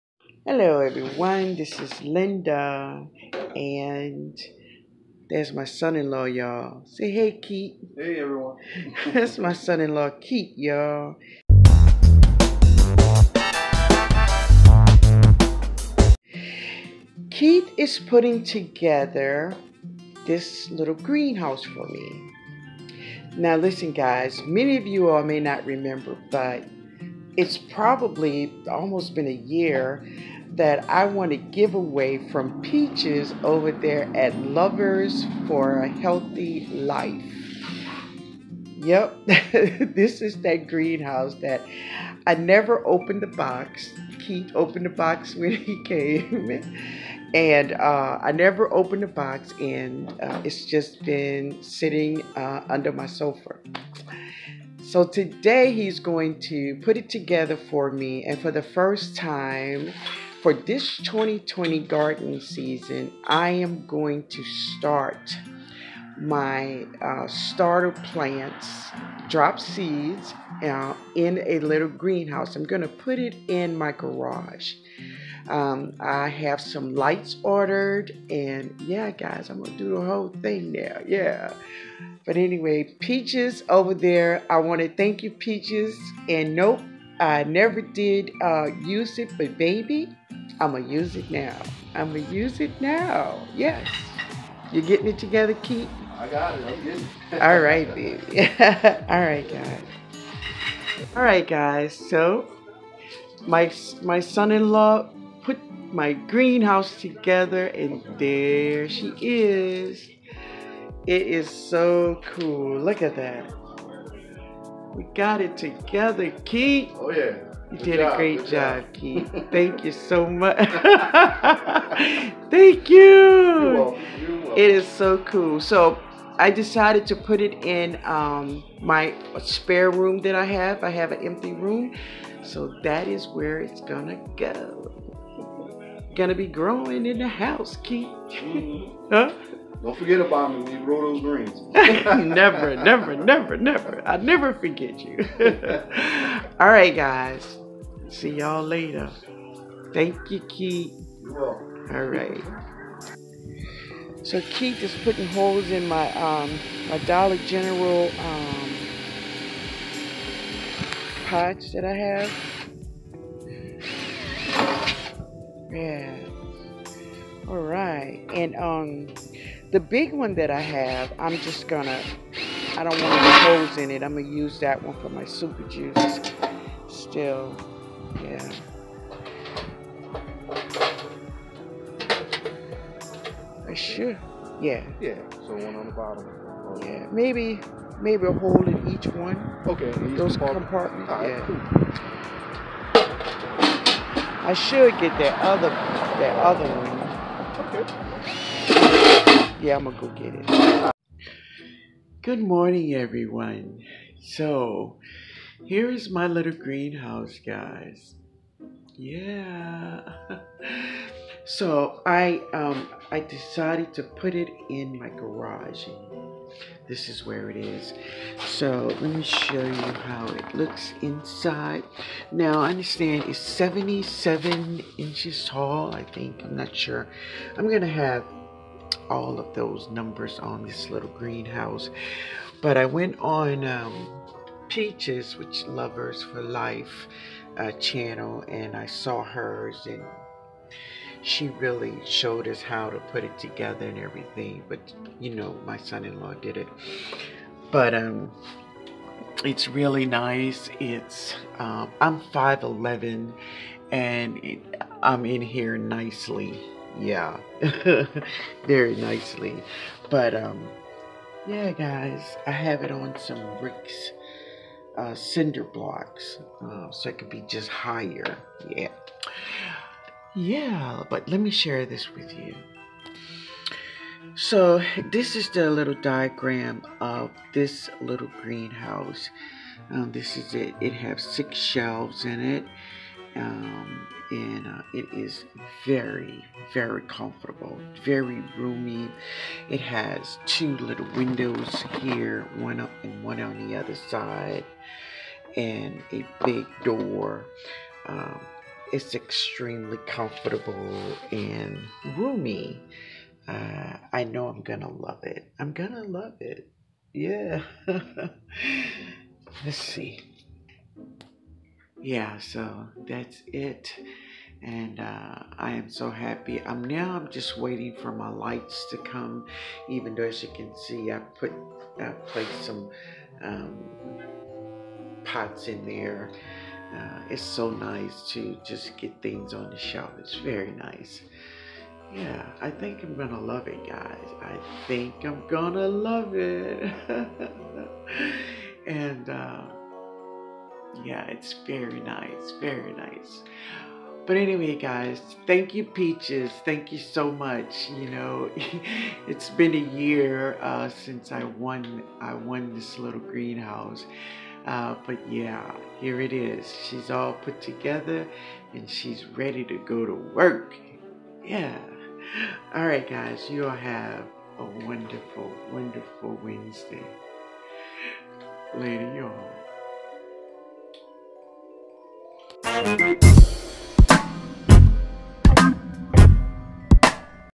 My First Little Greenhouse
Good Morning Everyone In today's video I'm Sharing My First Little Greenhouse, A Year ago I was one of the Winners of Peaches of Lover for a Healthy life, Peaches Giveaway was this little Greenhouse,